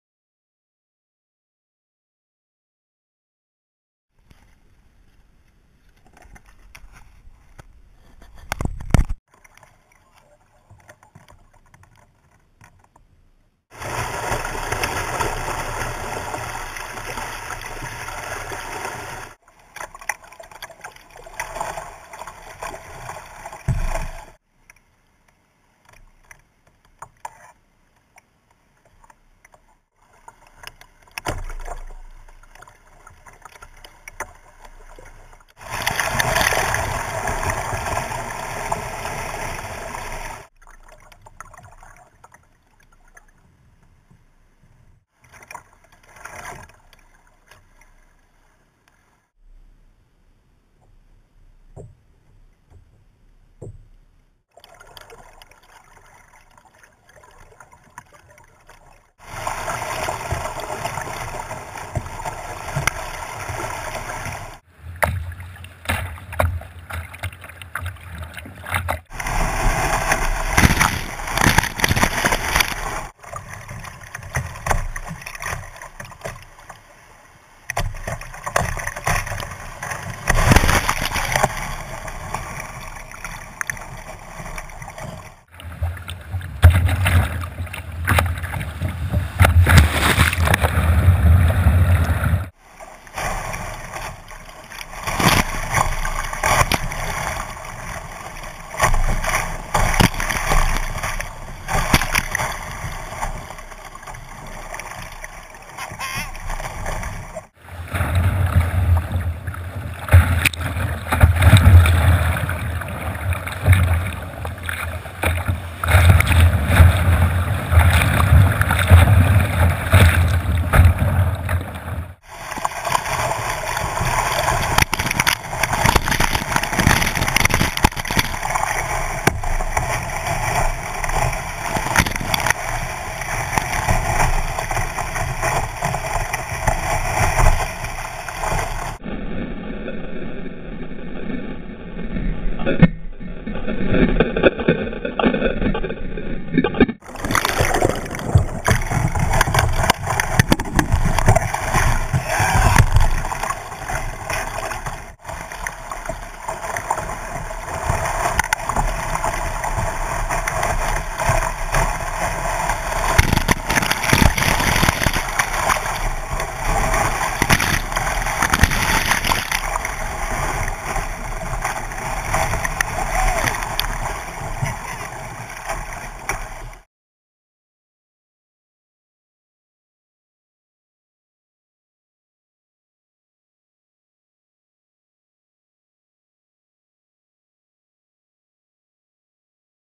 Kayak Fishing Stanislaus River
Dan from Headwaters Kayak shop brings you along for a day of kayak and fishing on the Stanislaus River from Knights Ferry to Orange Blossom.